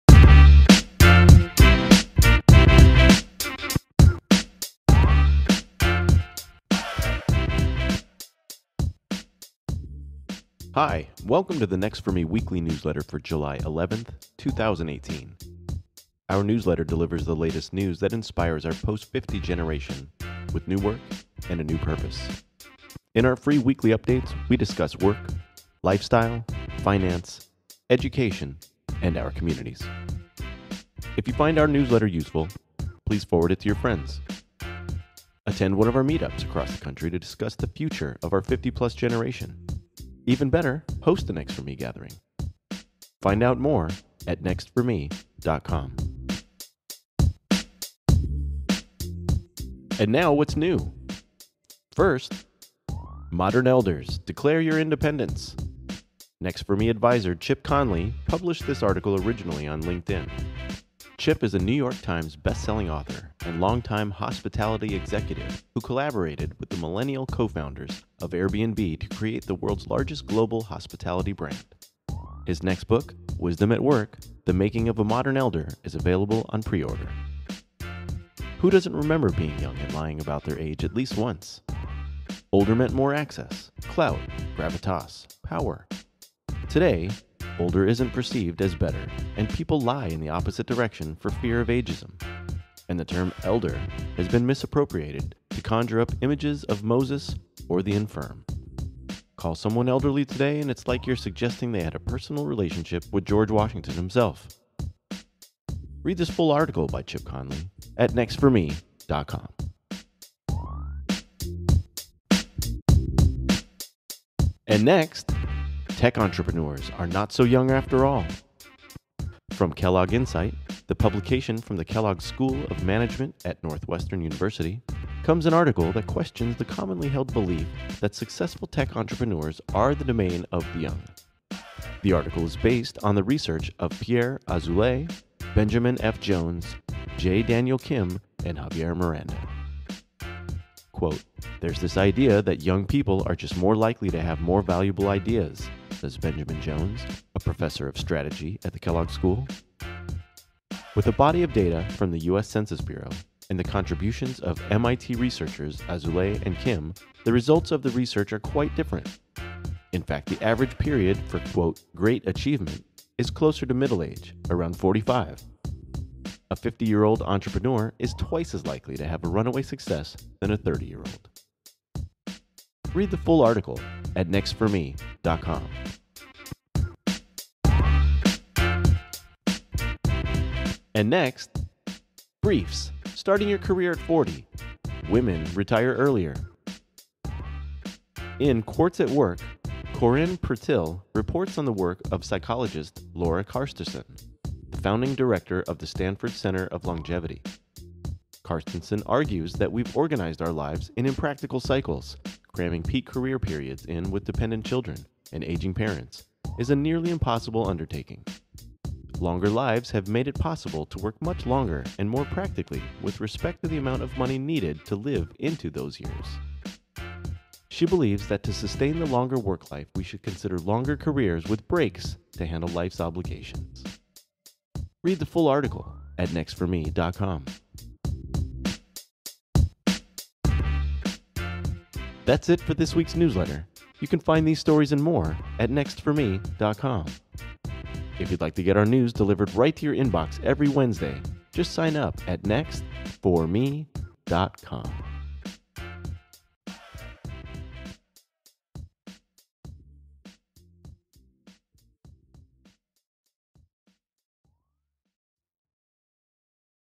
NFM Weekly Newsletter | Not So Young!
Welcome Back - Interdependence Is In!

This Week:
Modern Elders, Declare Your Interdependence!
Tech Entrepreneurs Are Not So Young After All
Briefs: Starting Your Career at 40, Women Retire Earlier

We learn a lot about life after 50 from our readers. At our events we discuss work, our communities, and of course what’s next.

Attend a Next For Me event.

If you enjoy our newsletter, please forward it to your friends.